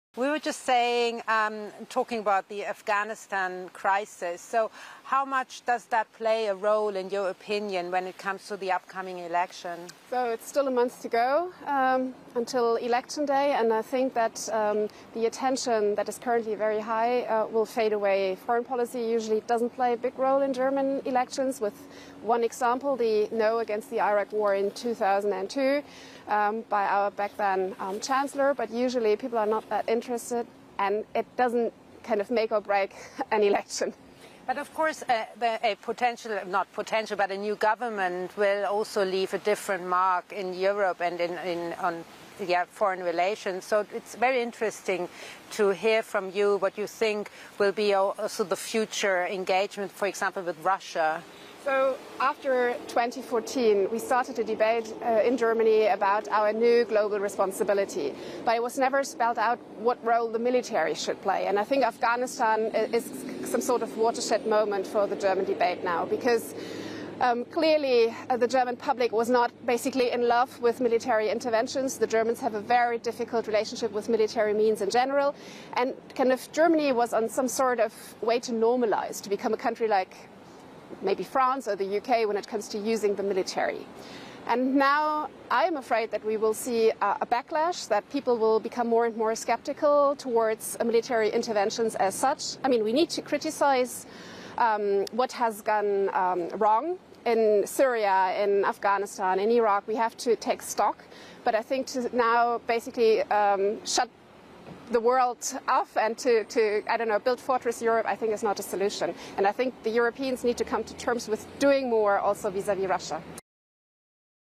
Afghanistan is a watershed moment for Germany's international relations, expert says
Jana Puglierin, senior policy fellow & head of ECFR Berlin, speaks to Annette Weisbach about the unfolding Afghanistan crisis ahead of the German elections.